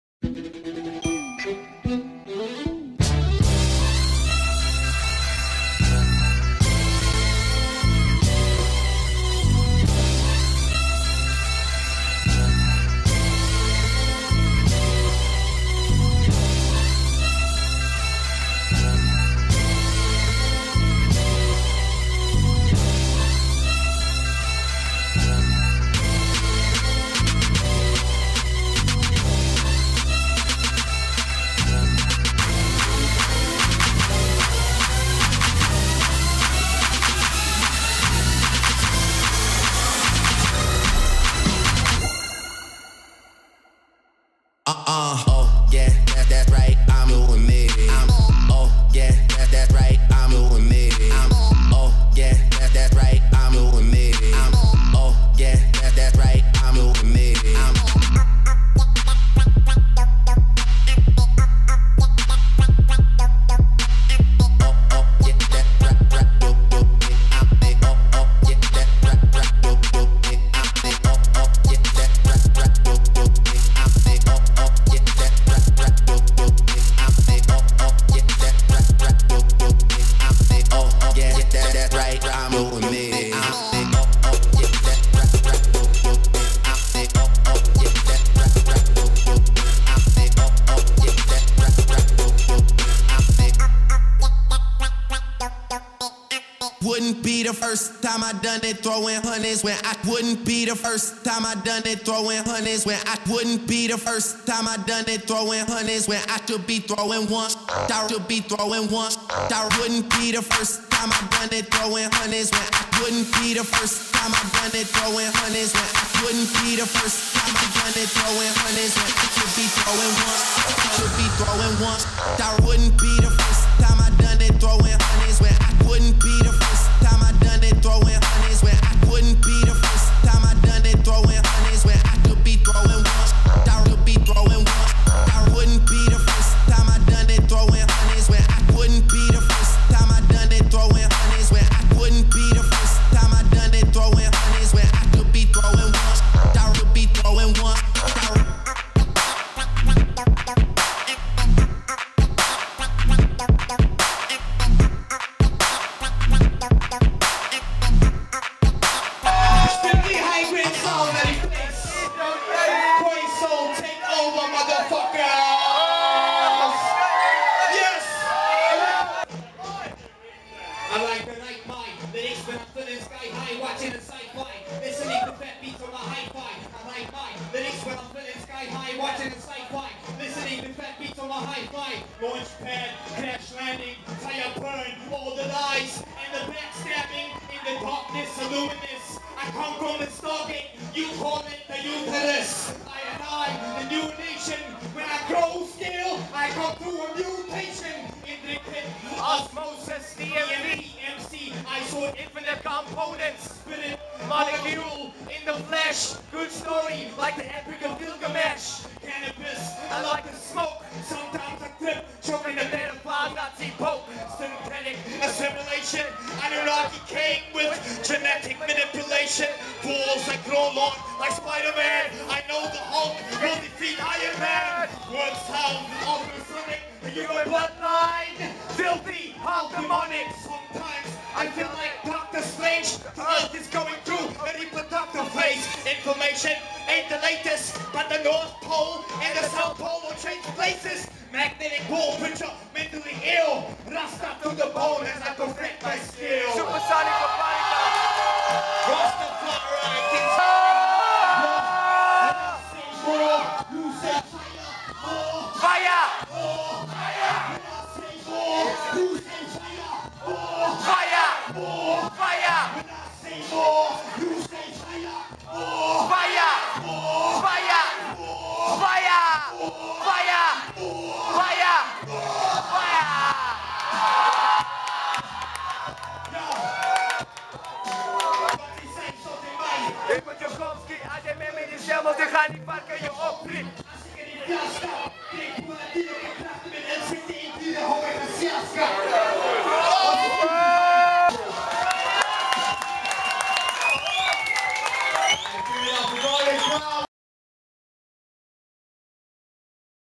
KHOISOUL TAKEOVER at Cold Turkey 25 May 2014
A few Snippets of the Khoisoul Takeover on 25 May at the Train Lodge in Cape Towns CBD featuring Garlic Brown, Clint Abrahams, JYD and Terrasoul